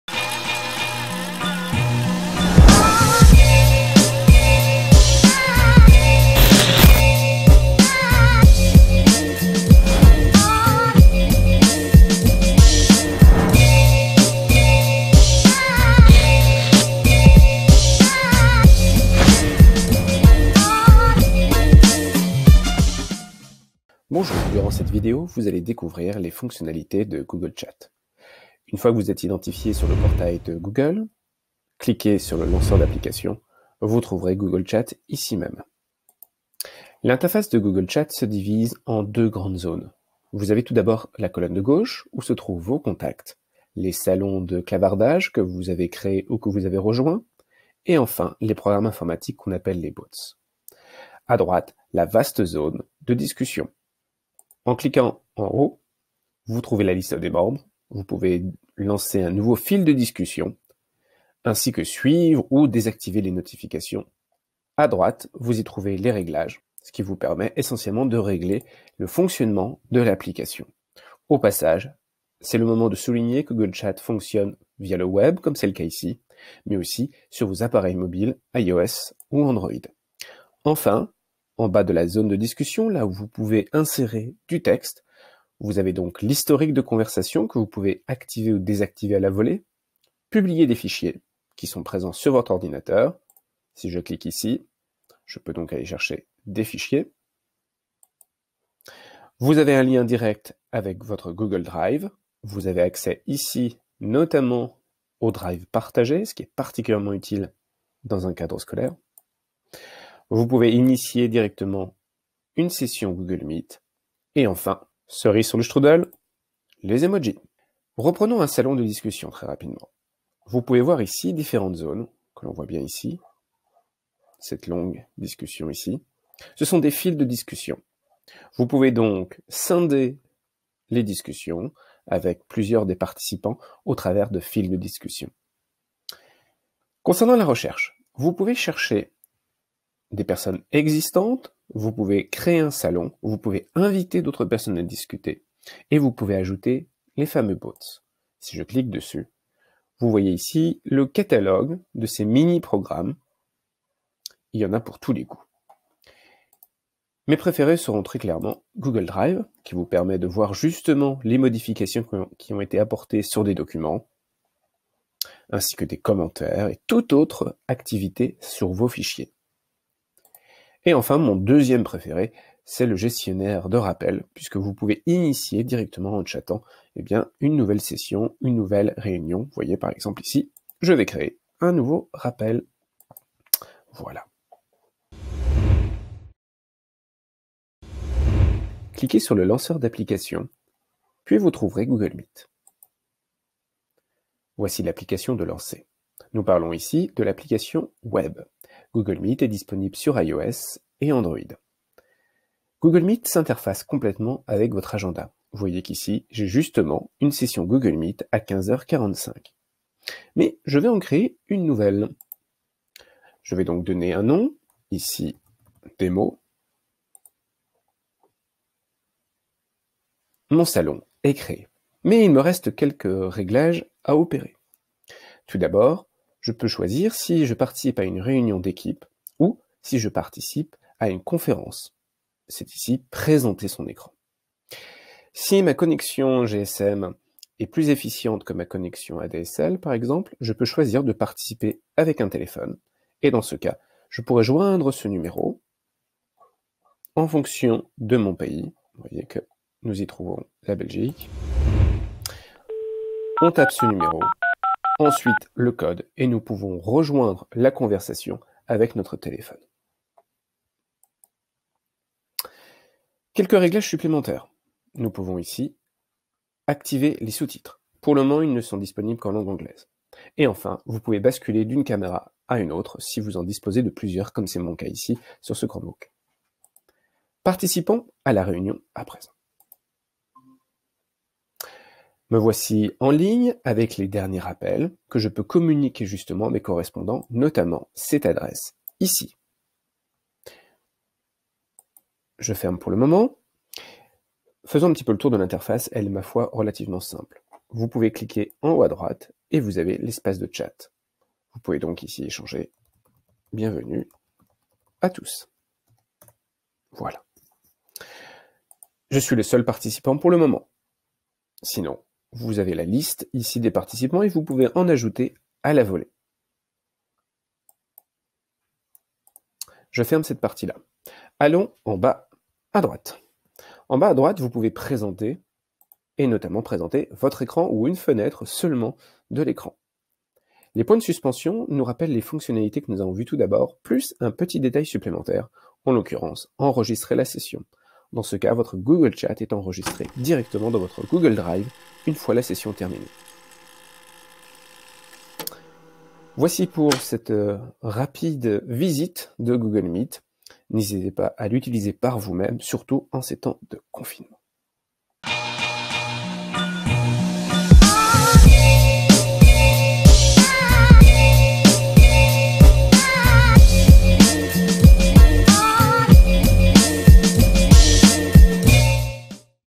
Tutoriel 2 : Google Chat et Meet
Découvrez dans ce tutoriel, comment utiliser les outils Google Chat et Meet !

Présentation réalisée par Ghislain Dominé, chef de projet numérique dans le réseau Canopé France.

Quelles sont les fonctionnalités de ces deux applications, et comment les utiliser facilement ?